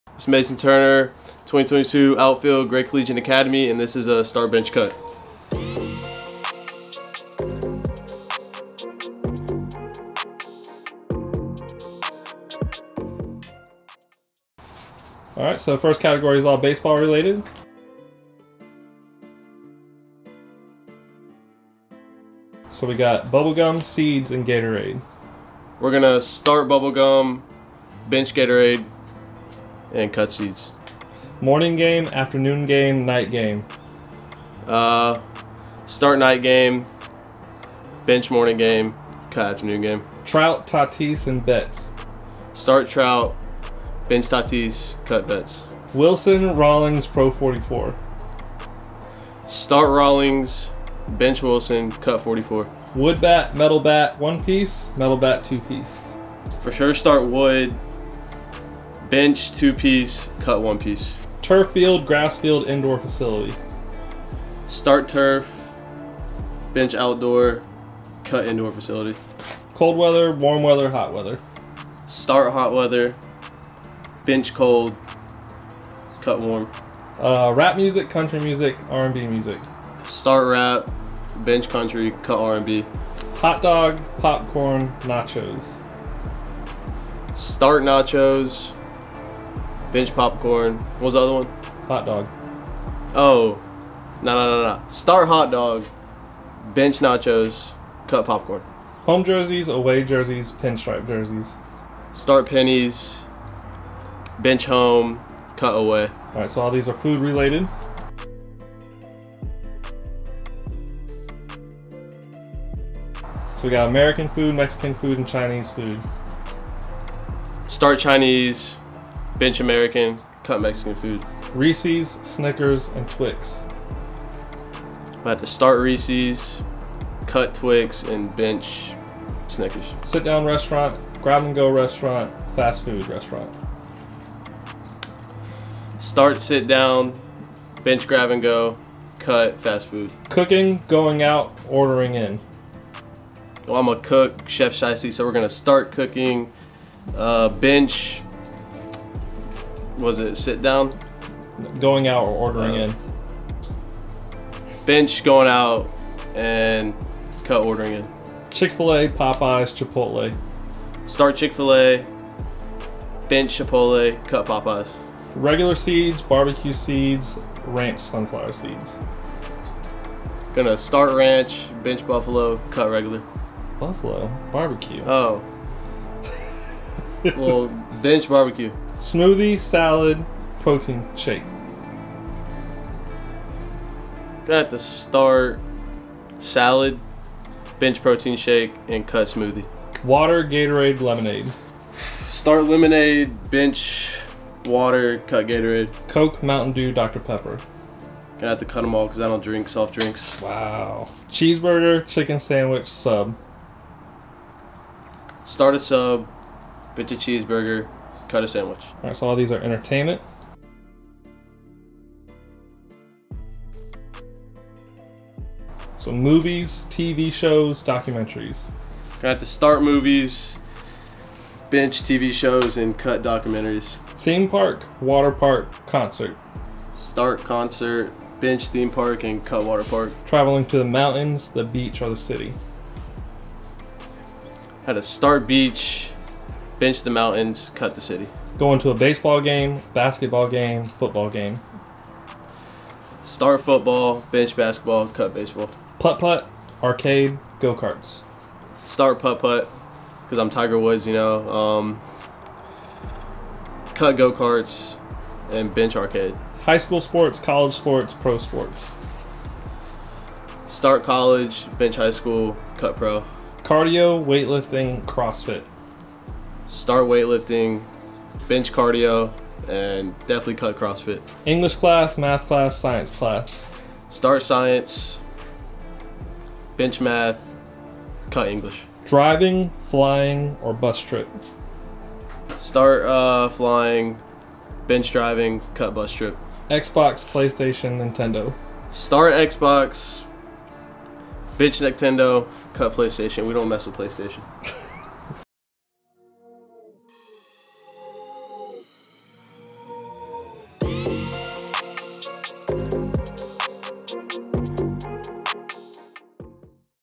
Start, Bench, Cut! FT. Mason Turner A Class Of 22' Outfielder Committed To Florence Darlington Tech
For This Episode I am down in Columbia South Carolina filming with Mason Turner a Class Of 2022 Outfielder who at the time of filming this was Uncommitted, but is currently committed to Florence Darlington Tech! We sat down as I throw him People, Places, and Food for him to start bench and cut, so you get to know this weeks athlete a little better! Be sure to give him a Follow on Instagram @Mason_.Turner & give me a Follow as Well @ShotByHype.

Please leave a Thumbs Up on the video as it helps grow the channel as Well!